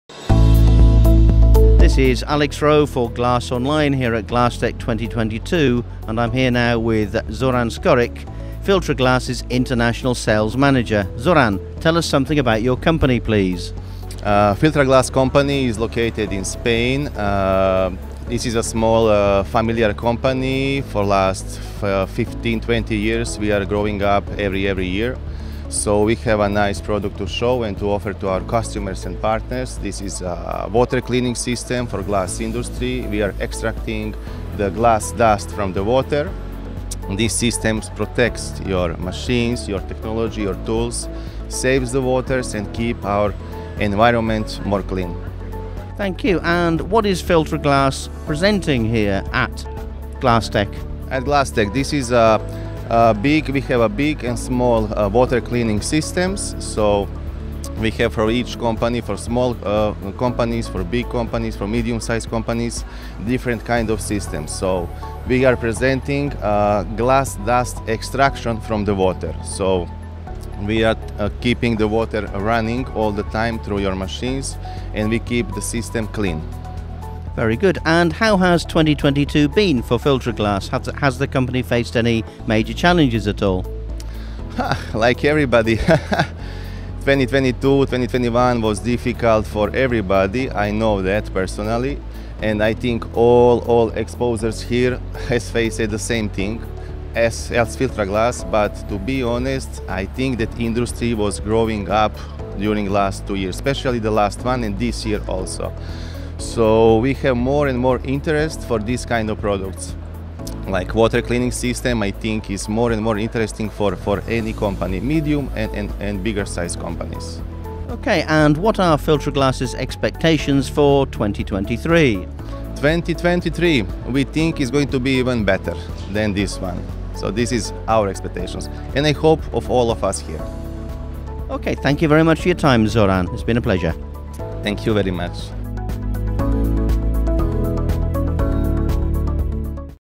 Video Interview with FILTRAGLASS | Glasstec 2022
Exclusive interview with Zoran Skoric, International Sales Manager of FILTRAGLASS
Glasstec 20-23 September 2022 | Düsseldorf, Germany

all the interviews made with the biggest players in the hollow and flat glass industry